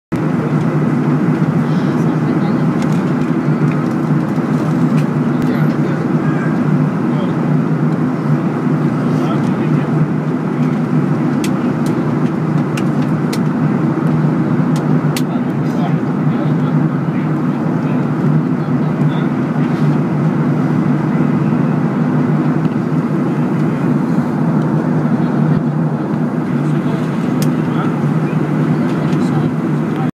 757 Inflight Full Moon
In flight from San Antonio,TX to Atlanta, GA over Louisiana at cruise altitude with a full moon rising in the east and the sun setting in the west. Video taken from my IPad.